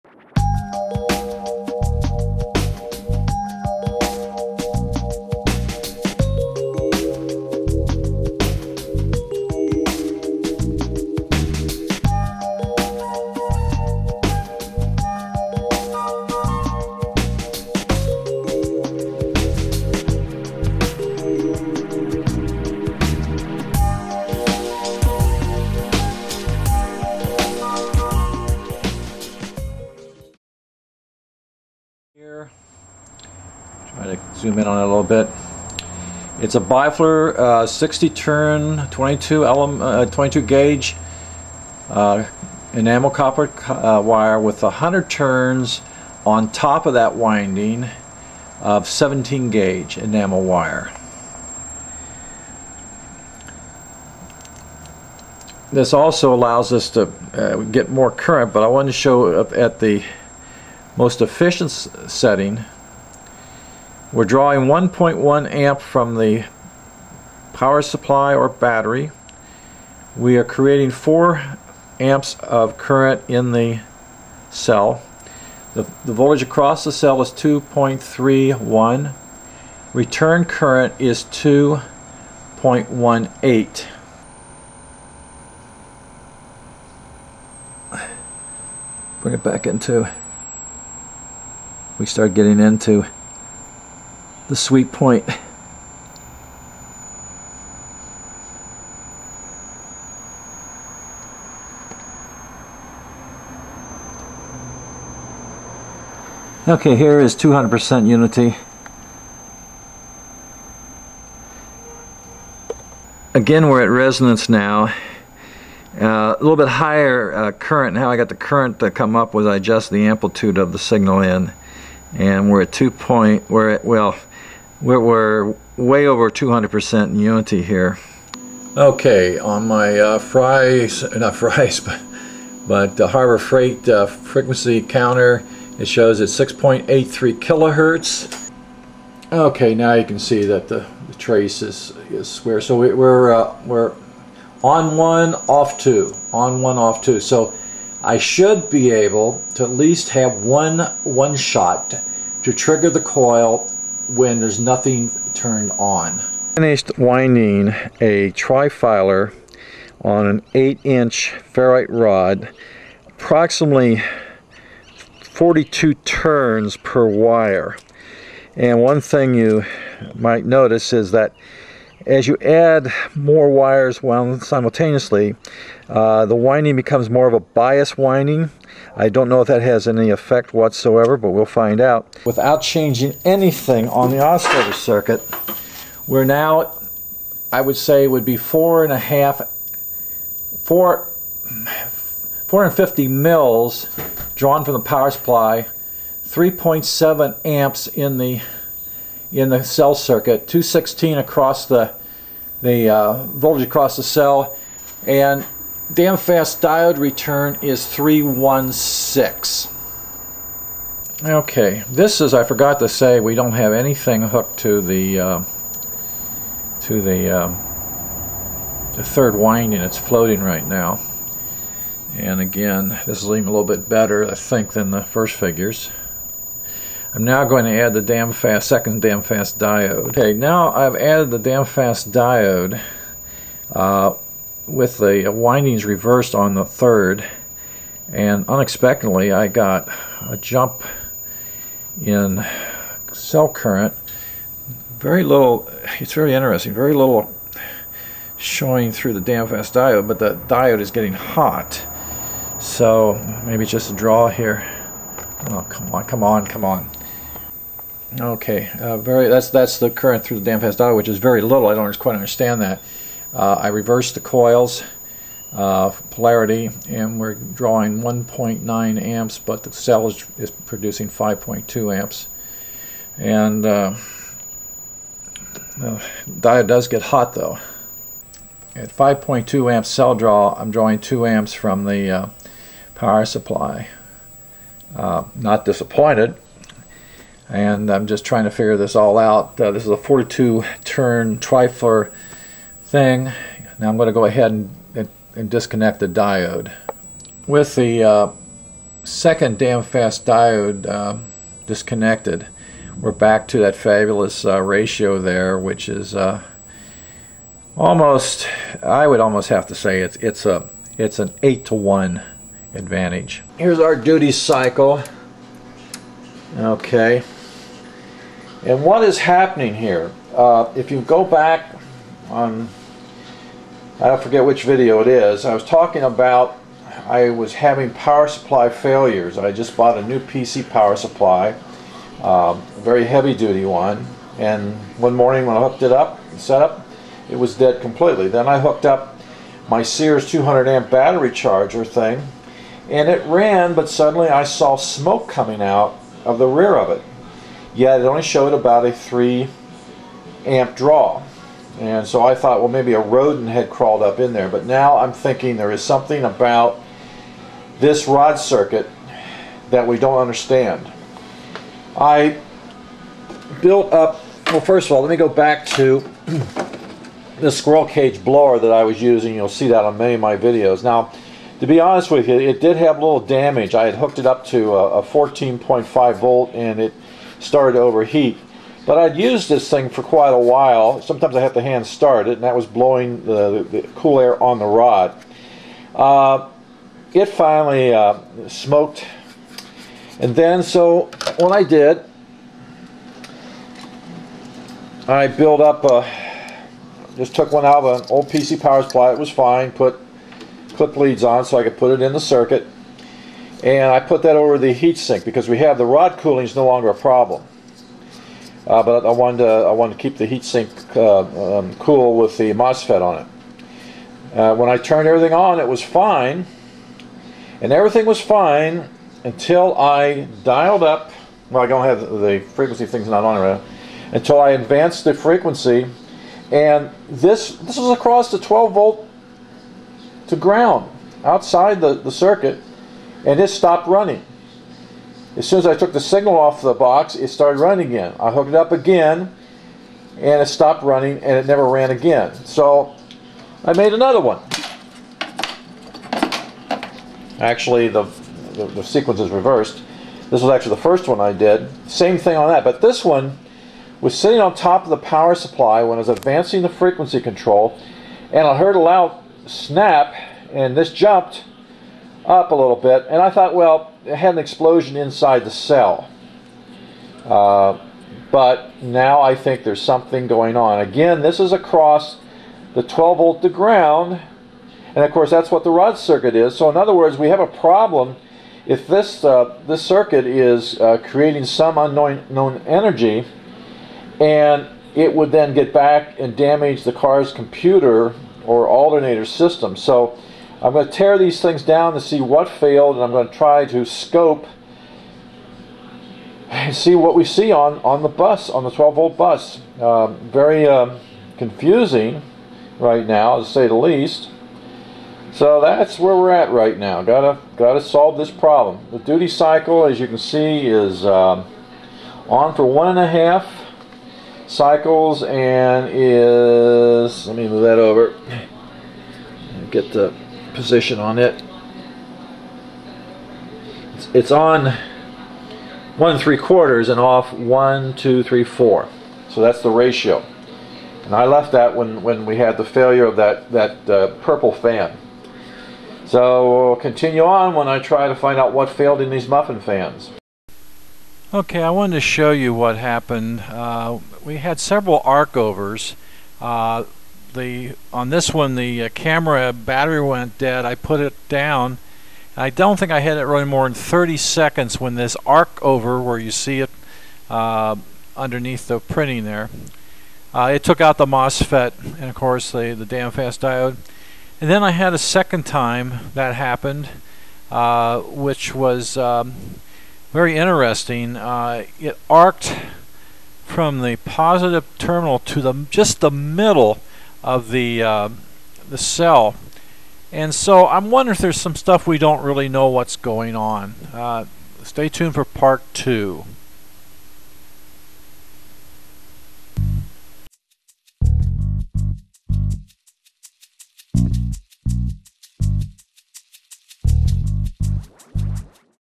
HHO Current Over Unity Research #1
This is the continuing research of current / power over unity "Rod Buck Voltage Convertor" It all started out to find a cheap way to convert 12 volts DC to 3.5 volts DC. Now it has evolved  in to reduce the drain from the cars's battery / alternator system to run HHO cells. In part 2 you will see 1200% current over unity. The trade off is 0.1 amp from the power source produces 1.3 amps fed to the cell. As the duty cycle is longer, the more current you will have but less current over unity. The way to have your cake and eat it too is to have very short duty cycles with the use of multi one shots driving either the same mosfet /  coil or several mosfet / coils circuits. There should not be any problem scaling this upward. There is another person using my circuit in the process of do that now.